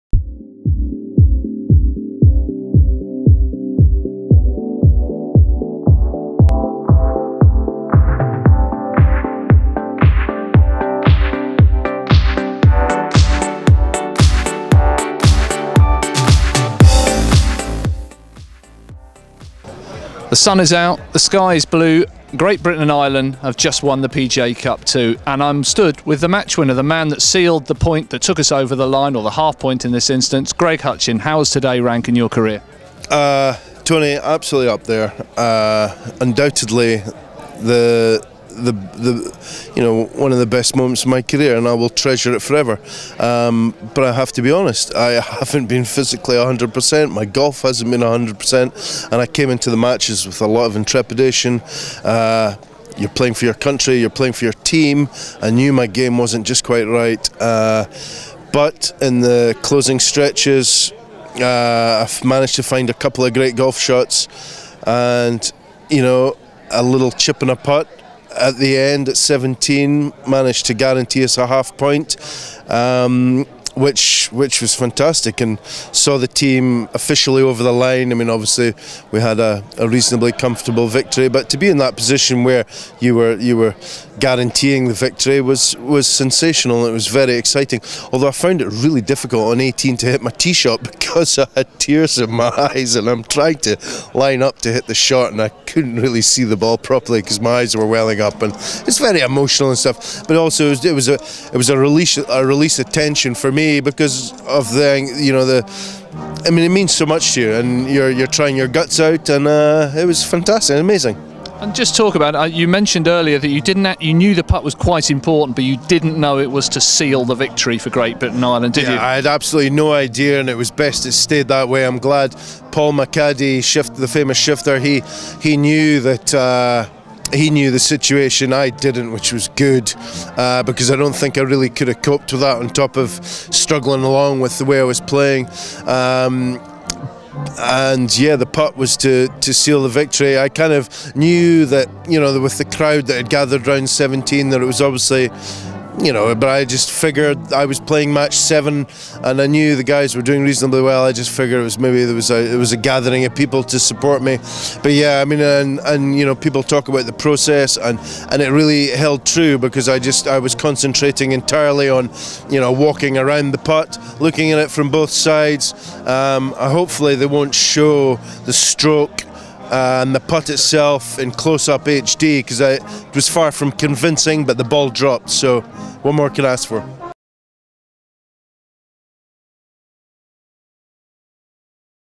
PGA Cup 2017 - Greig Hutcheon
Greig Hutcheon sunk the winning putt as Great Britain & Ireland beat America to win the 2017 PGA Cup at Foxhills Club & Resort.